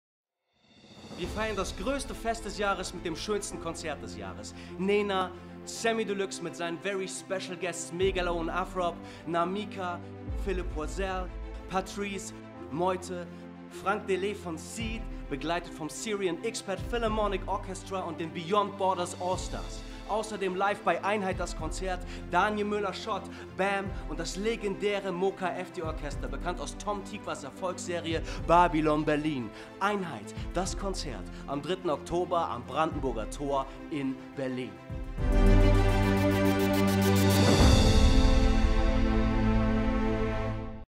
1HEIT - DAS KONZERT | LINE UP (Teaser)
Playlist | 1HEIT - DAS KONZERT

Am 3. Oktober 2018 feiern mit Euch Künstler*innen wie NENA, Samy Deluxe feat. Afrob + MEGALOH, Philipp Poisel, Patrice, Namika, MEUTE, Frank Dellé, Moka Efti Orchestra, Syrian Expat Philharmonic Orchestra - SEPO, Daniel Müller-Schott Official Site & BÄM! Drumline Berlin.

1HEIT – DAS KONZERT (2018) setzt gemeinsam mit den Musiker*innen, die auf der Bühne stehen, ein unüberhörbares Signal für gesellschaftliches Engagement, Partizipation, Gleichheit und Toleranz. Die Helden der 1HEIT sind vor allem diejenigen in unserer Gesellschaft, die sich mit ihrem Engagement für gesellschaftlichen Zusammenhalt einsetzen.

Das Konzert präsentiert präsentiert stellvertretend für die vielen Bürgerinnen, die sich mit ihrem Engagement für gesellschaftlichen Zusammenhalt einsetzen, drei Projekte, die mit ihrem Engagement ihr Umfeld, ihre Heimat und die Welt positiv beeinflussen – die Helden der 1HEIT.

Unter dem Motto “Nur mit Euch” richtet das Land Berlin im Rahmen seiner Bundesratspräsidentschaft 2018 die offiziellen Feierlichkeiten zum Tag der Deutschen Einheit aus. Kulturprojekte Berlin GmbH konzipiert und realisiert im Auftrag des Landes Berlin das Bürgerfest und die begleitenden Kommunikationsmaßnahmen.

Ausgewählt wurden die Projekte von den drei 1HEIT-Kuratoren Fetsum Sebhat und Tedros Tewelde (Peace X Peace Embassy) und Björn Döring (Fachhandel für Ereignisse) in Kooperation mit der Onlineplattform Musik Bewegt.

Video Production, Recording and Edit by The Goodguys Entertainment GmbH

Sprecher: Lorris Andre Blazejewski